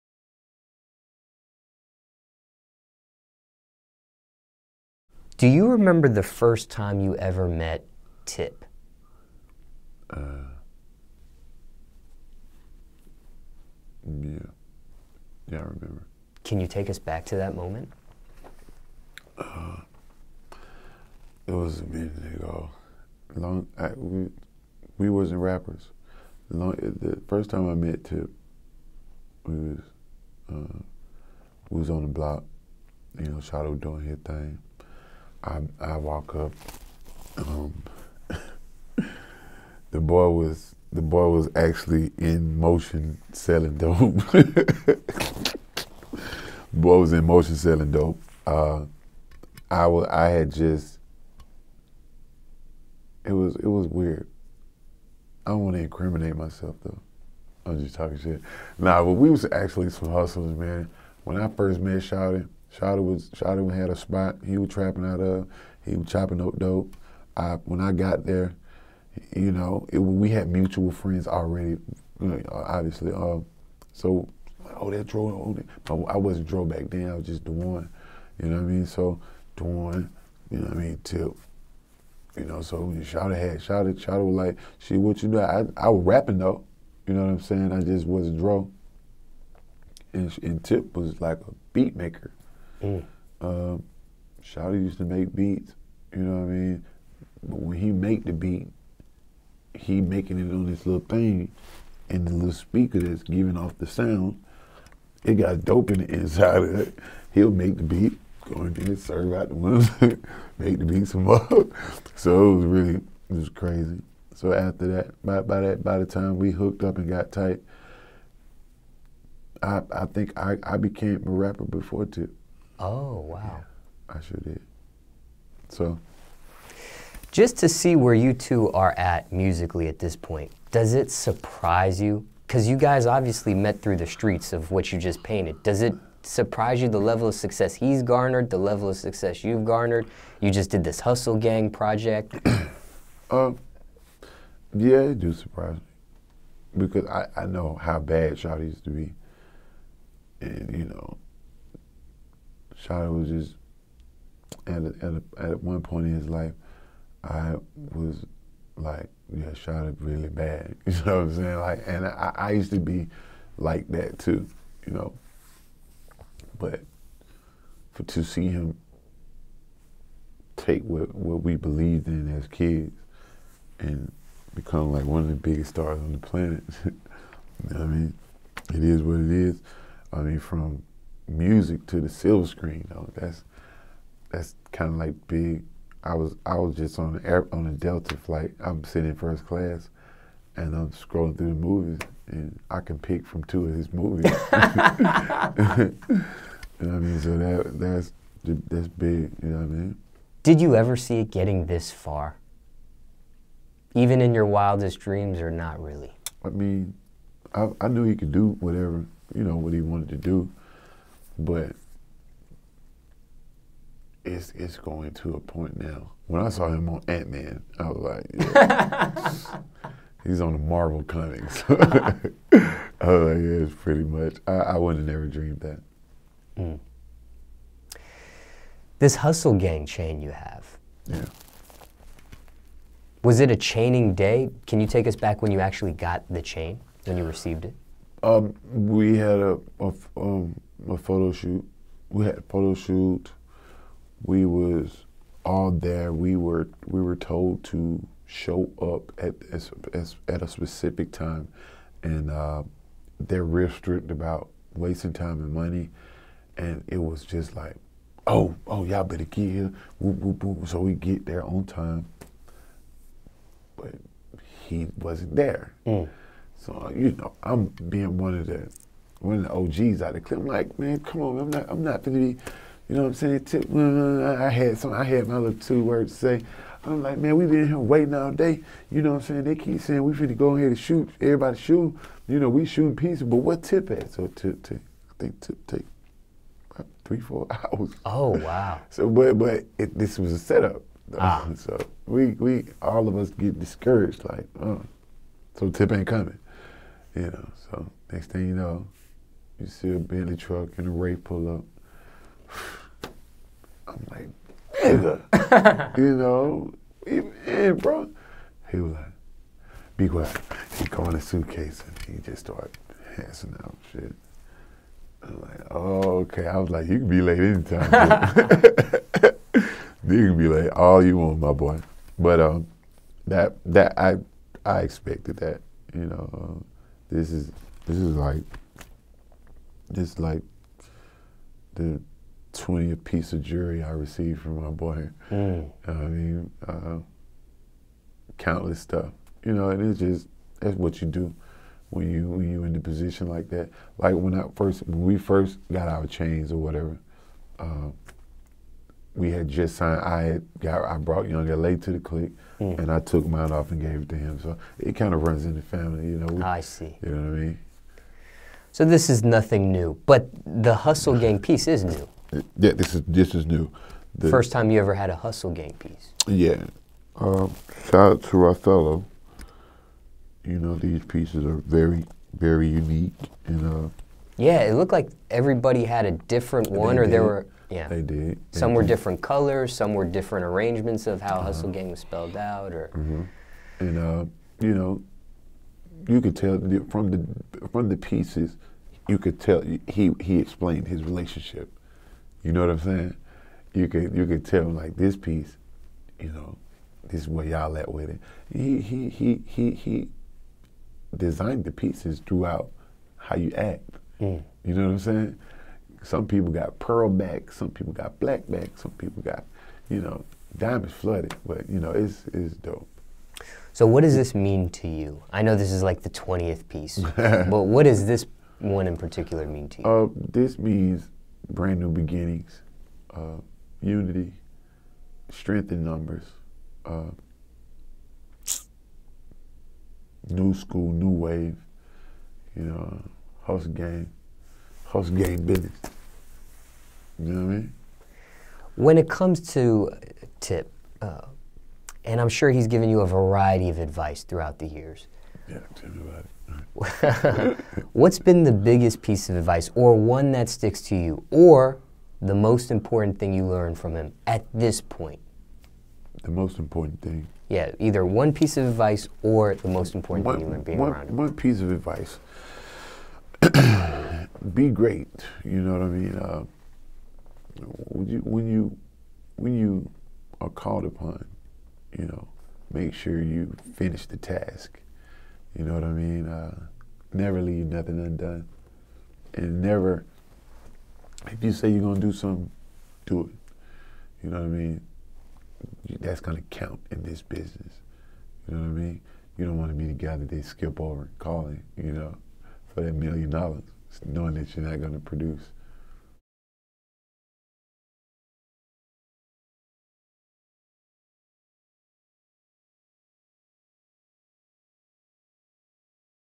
Young Dro Reflects On First Time Meeting T.I. and Receiving Hustle Gang Chain From Tip
Atlanta rapper and Hustle Gang member, Young Dro, sits down with DJ Smallz and reveals how he linked with the Hustle Gang, his chaining day and the biggest advice he received from its leader, T.I.